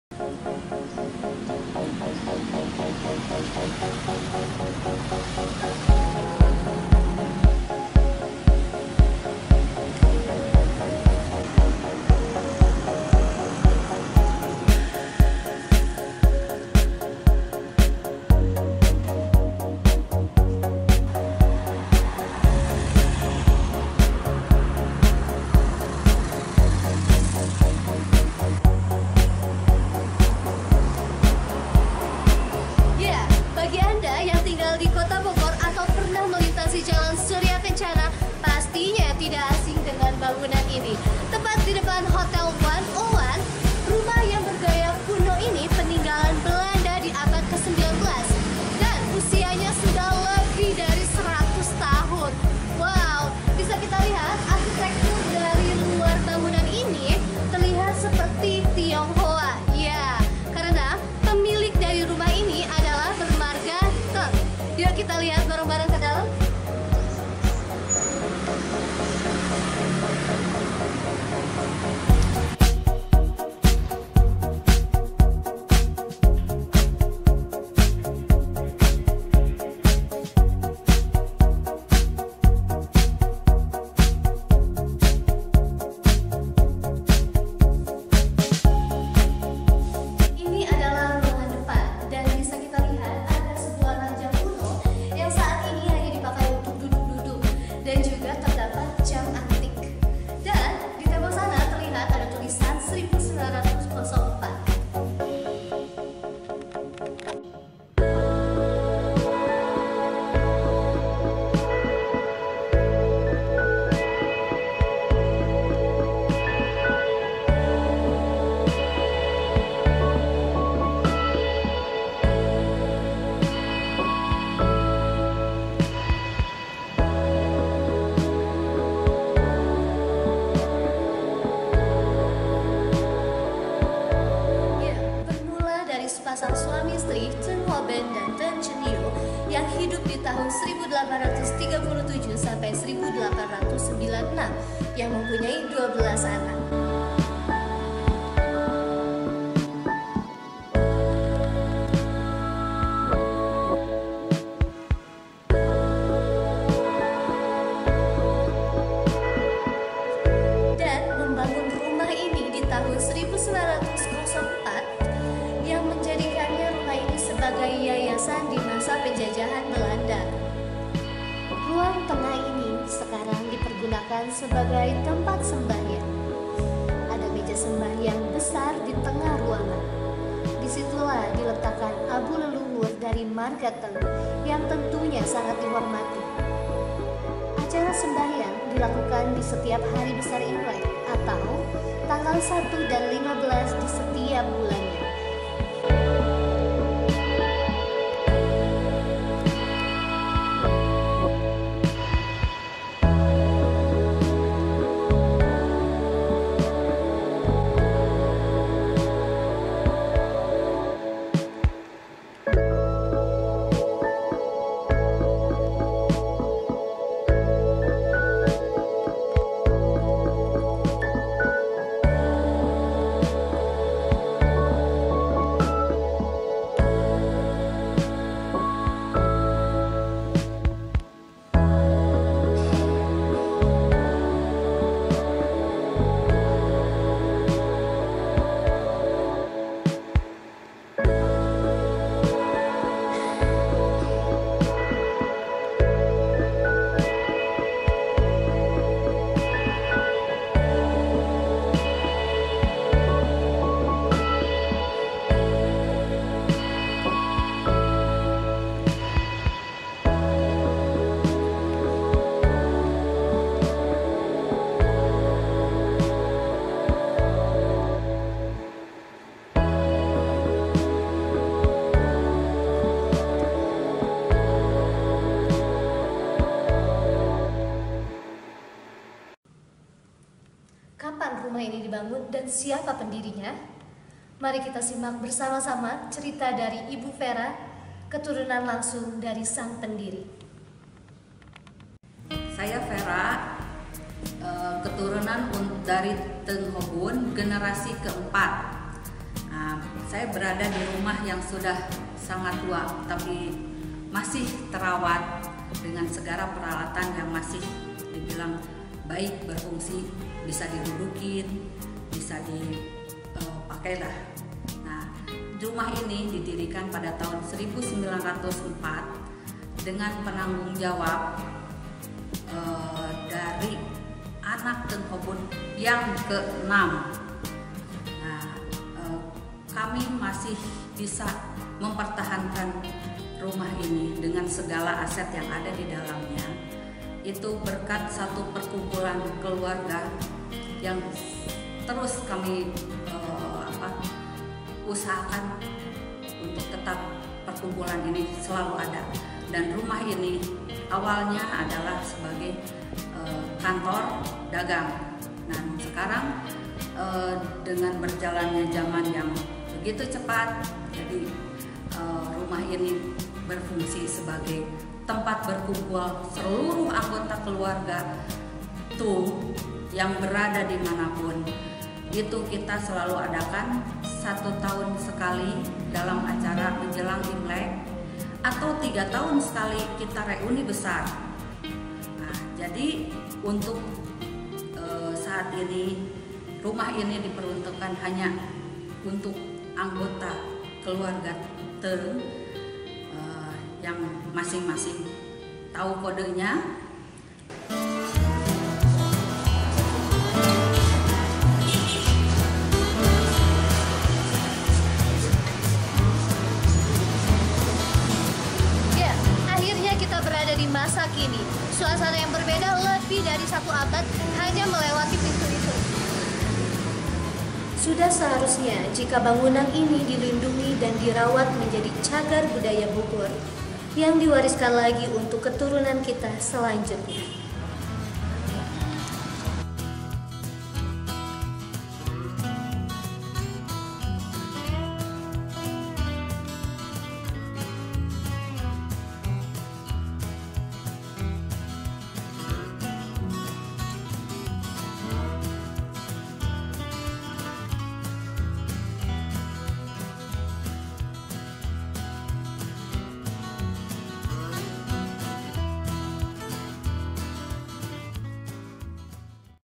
Rumah cagar budaya bersejarah di Jl Suryakencana - Bangunan kuno tahun 1904 di Bogor - Gedung KST
Bagi anda yang tinggal atau pernah di Bogor pastilah suatu ketika melintas di jalan ini , jalan Suryakencana.
Saat anda melewati jalan ini tentulah anda pernah melewati rumah ini.